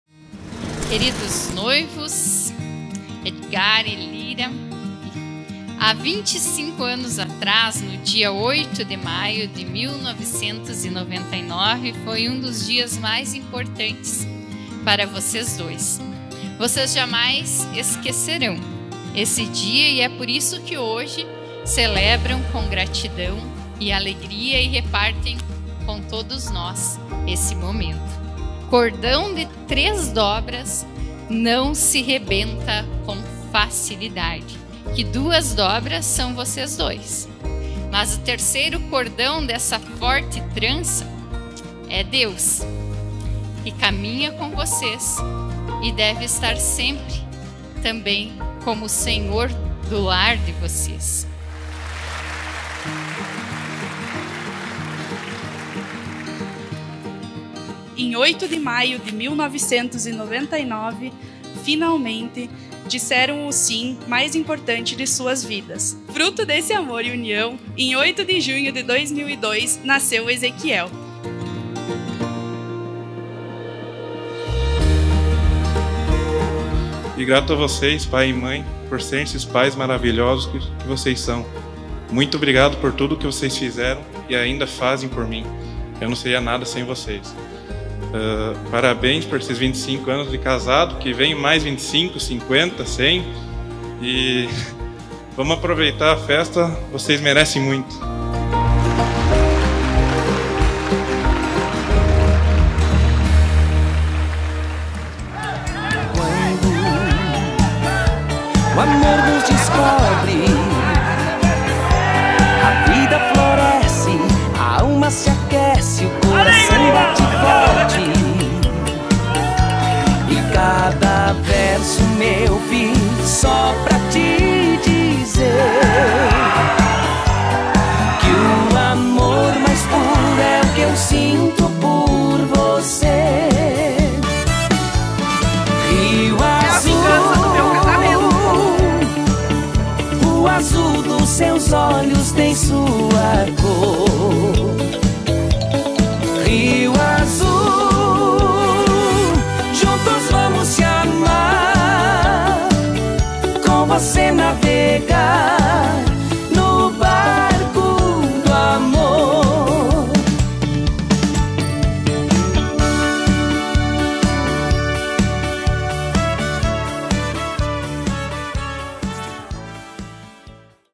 Bodas de Prata Edgar e Lira
Celebramos os 25 anos de amor e união de Lira e Edgar com uma linda festa de Bodas de Prata. Este vídeo captura os momentos especiais desse dia inesquecível, repleto de emoção, alegria e carinho. Que esses 25 anos sejam apenas o começo de uma jornada repleta de ainda mais amor e cumplicidade.

Aperte o play e emocione-se conosco!